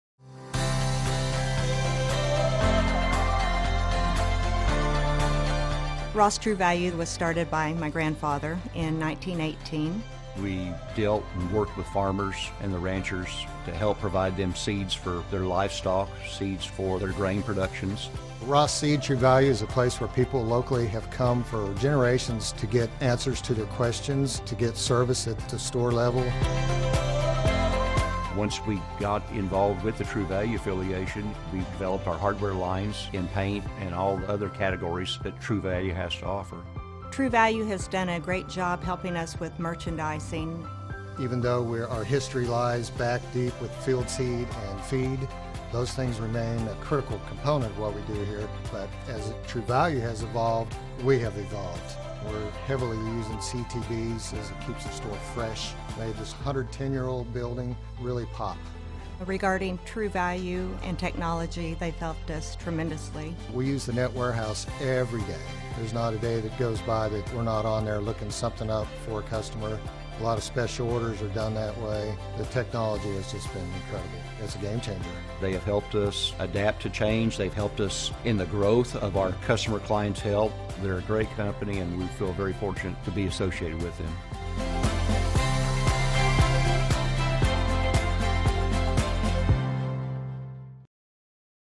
True Value Century Club Video
Our store was showcased in a nationwide campaign lauched by TrueValue as one of 5 TrueValue Hardware stores that have been in business for over a century. The clip provides a little more insight to 'Our Story'.